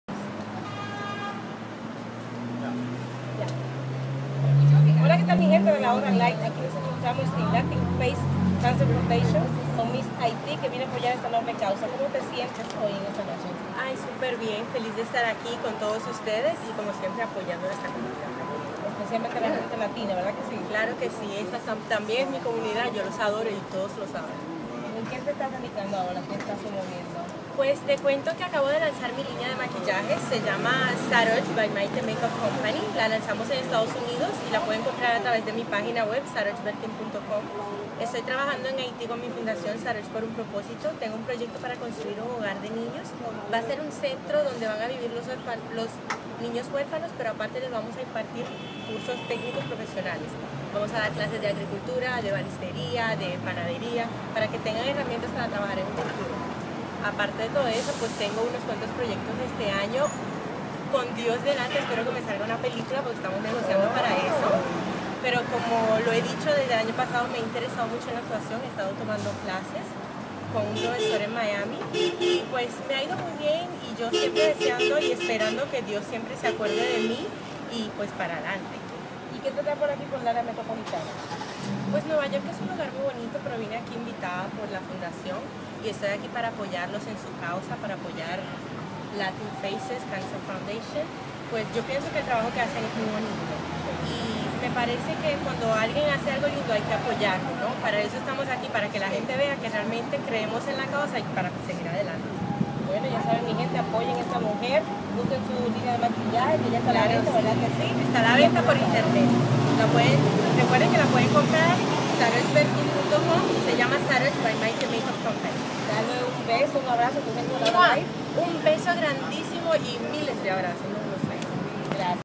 Entrevista a Sarodj Bertin, en la gala Caminando por un manana libre de Cancer
Sarodj en la alfombra de la gala a beneficio de la 2da caminata latina contra el cancer, que este ano se llamara, "Caminando Por Un Manana Libre De Cancer"